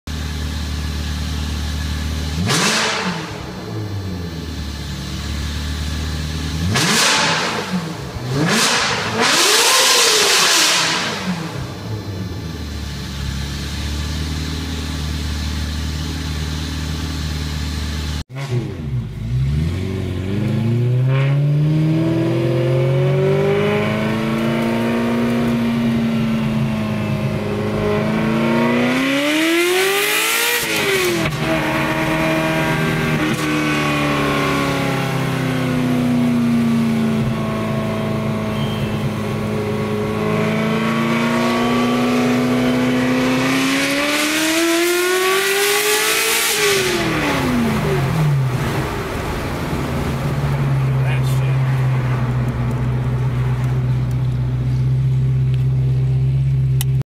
Ferrari 348 Spider with F1 sound exhaust
Ferrari 348 Spider with new F1 sound exhaust. full open setup.